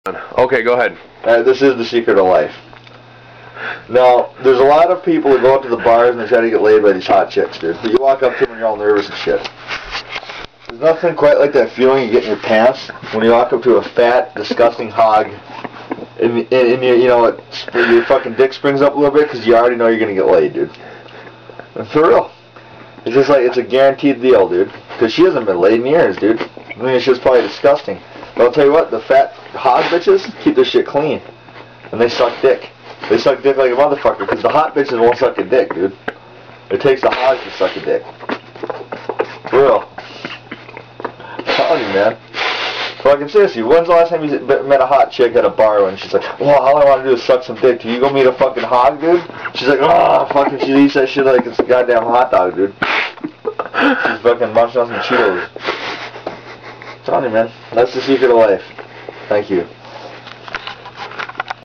"The Secret of Life"
My friend Ray explains the secret of life, in a very short period of time.